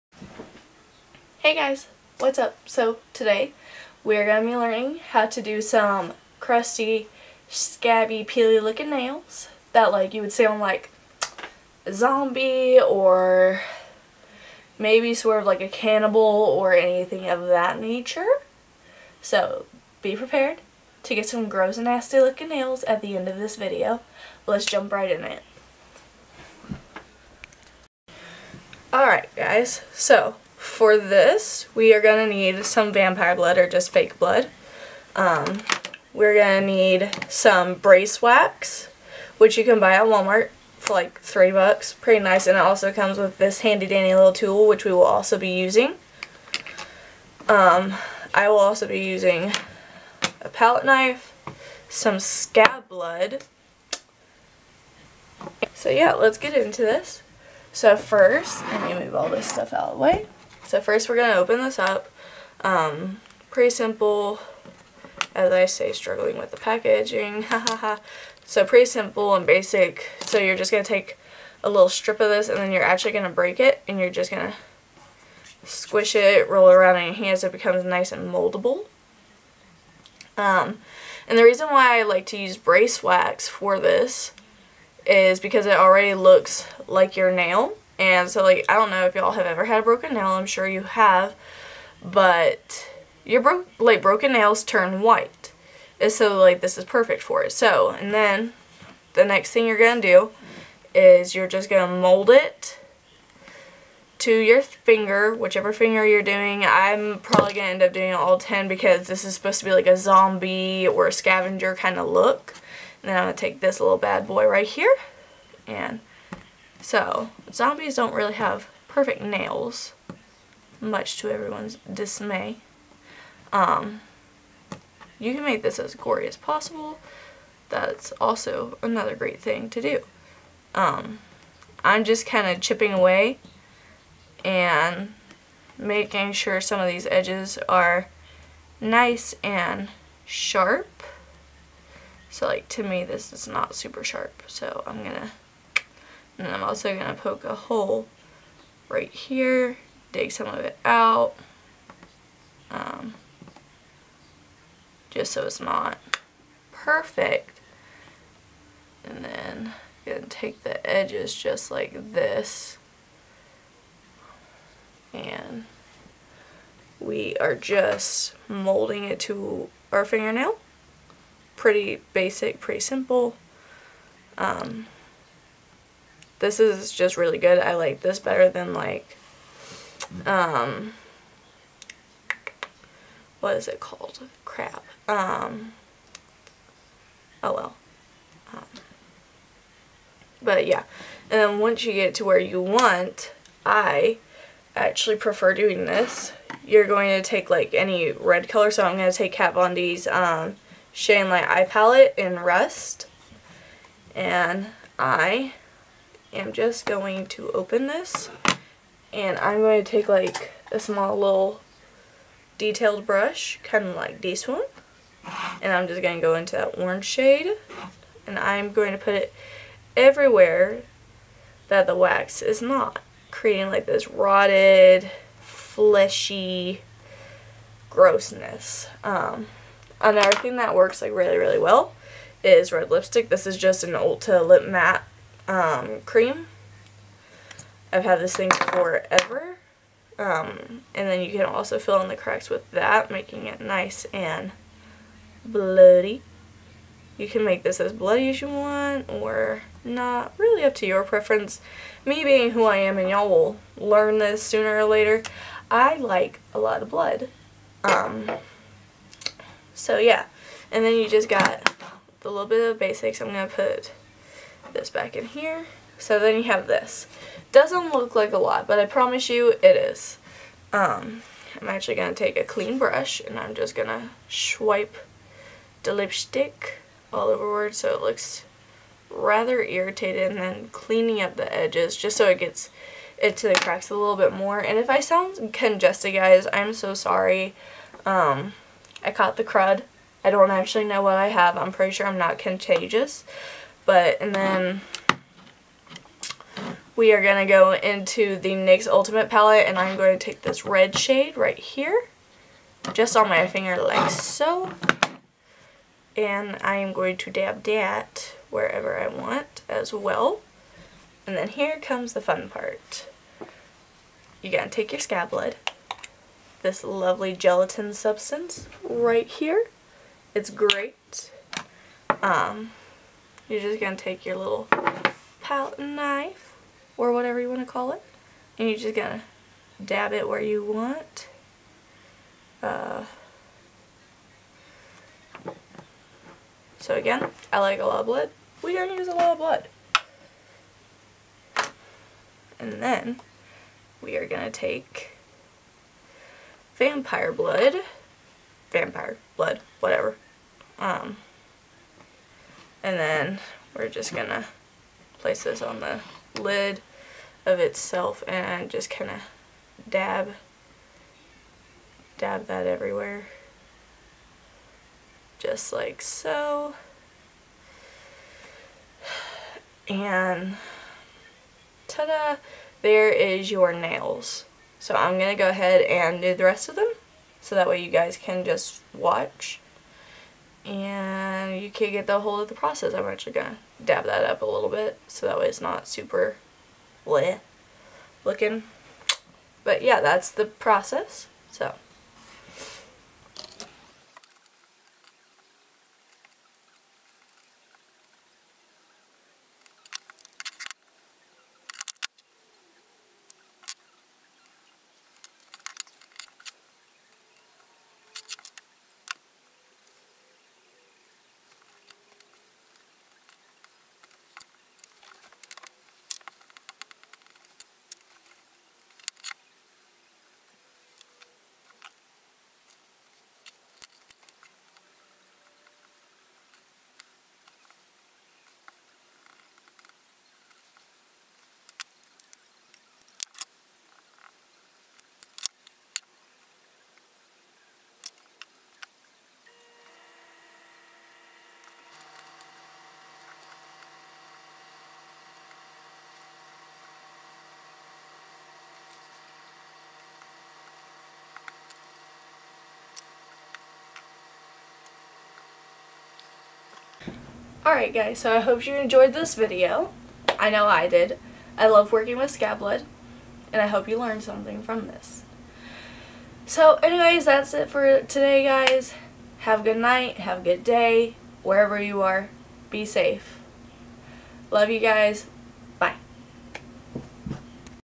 Zombie Nails | Special Effects Makeup
add this to your zombie costume for more realistic hands!